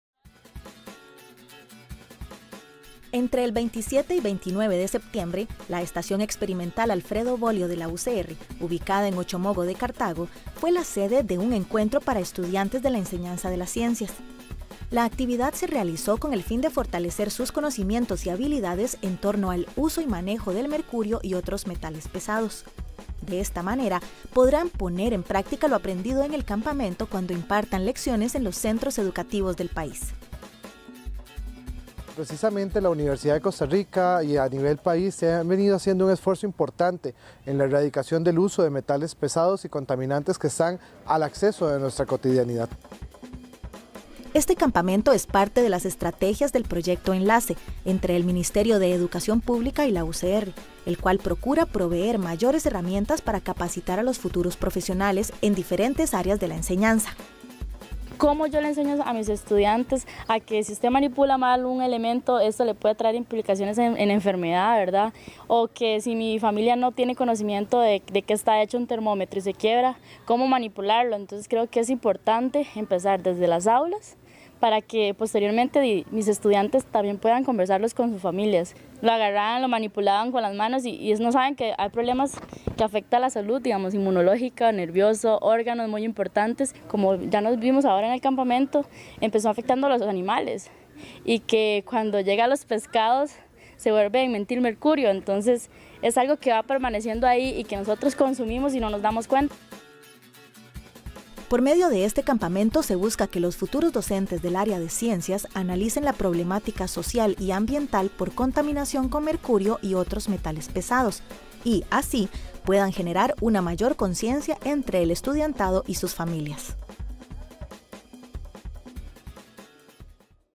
UCR y el MEP unen esfuerzos contra el uso del mercurio y otros metales pesados
El Campamento didáctico para el aprendizaje de la química y sus implicaciones medioambientales es una capacitación dirigida a estudiantes de la enseñanza de las ciencias de las universidades públicas.
Esta actividad se realizó gracias al Proyecto ENLACE: una iniciativa conjunta entre el Ministerio de Educación Pública y la UCR.